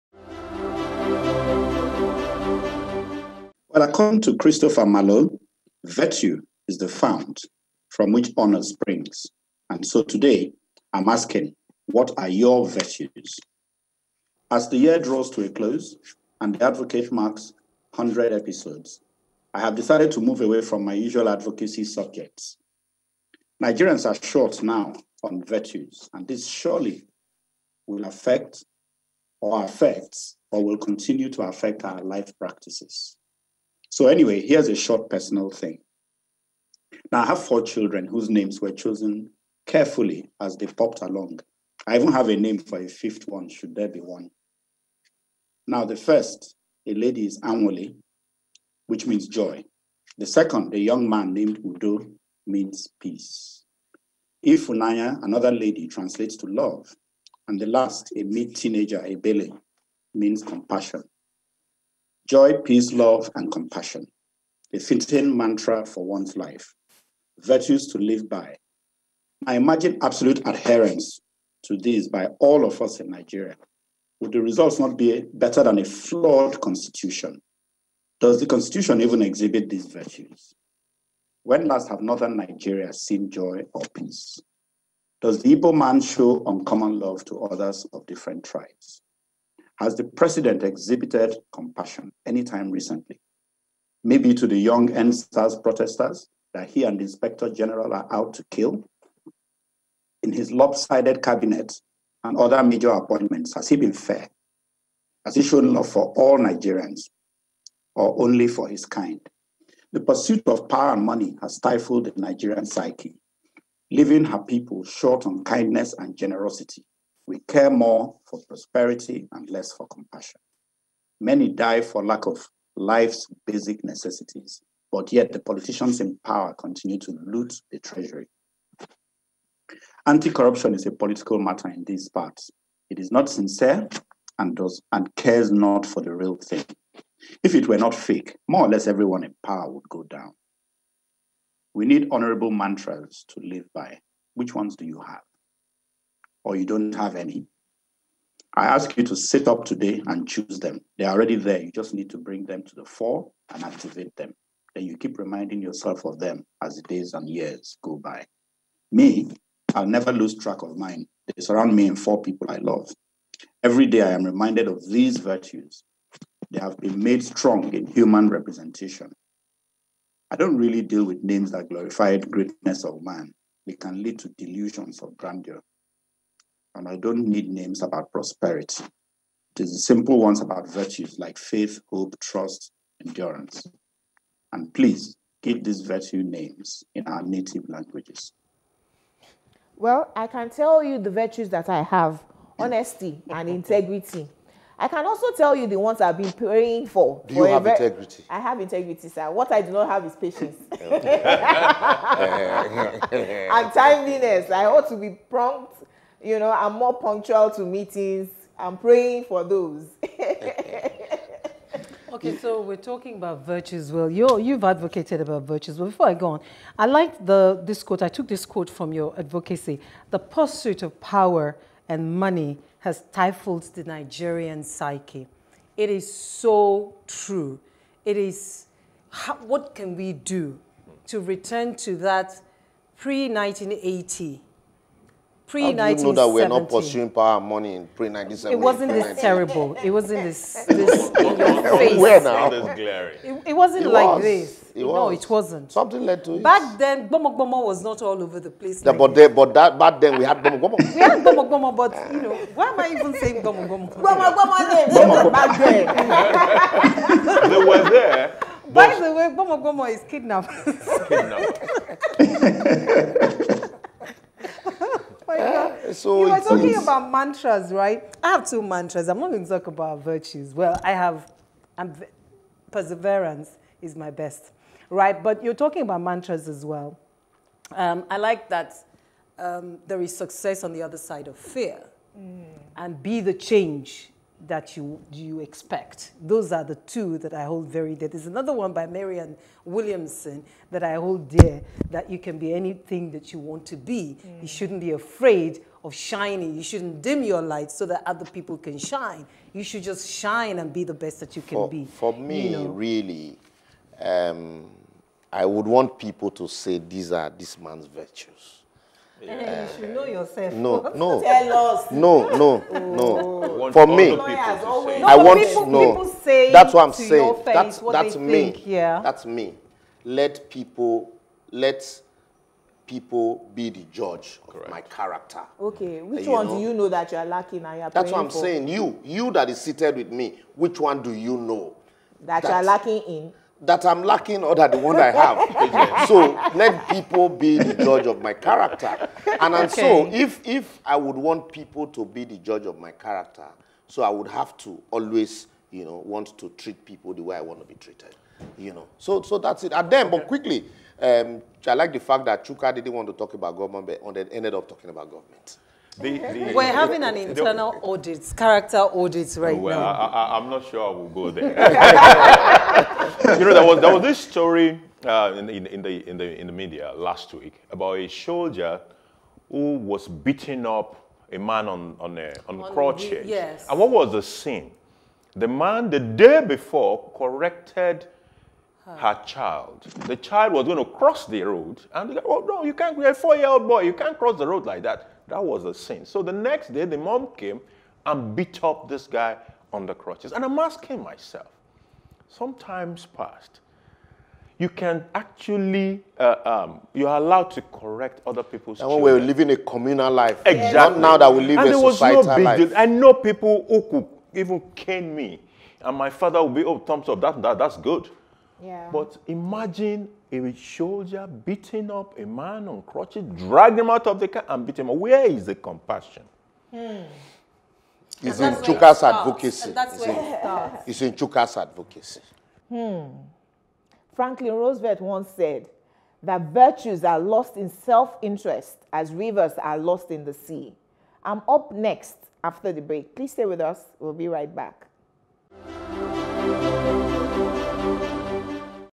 What Are Your Virtues? | TheAdvocateNG EP 100
The pursuit of power and money has stifled the Nigerian psyche, leaving her people short on kindness and generousity. We care more for prosperity and less for compassion. Many die for lack of life’s basic necessities, but yet the politicians in power continue to loot the treasury.